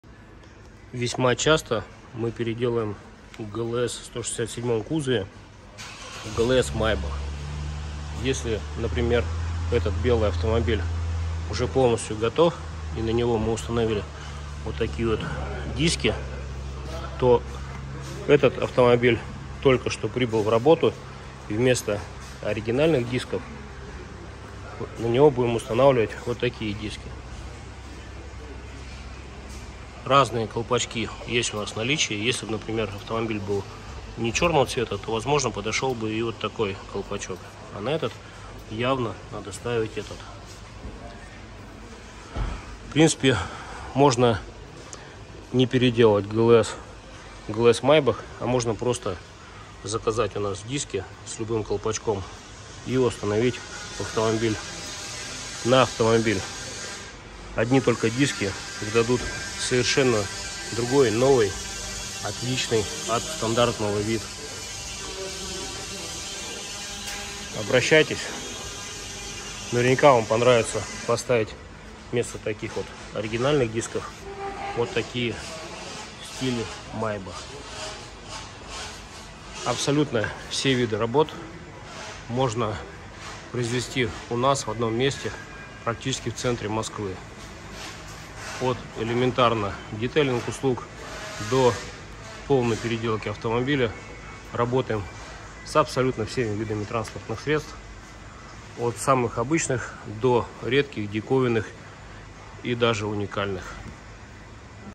Подбираем диски при переделке из GLS 167 в GLS Maybach со склада или под заказ
Если Вам нужны диски для авто, то просто приезжайте и мы подберём вместе из наличия или привезём под заказ

Любые виды работ по доведению состояния Вашего авто до идеального в одном месте практически в Центре Москвы в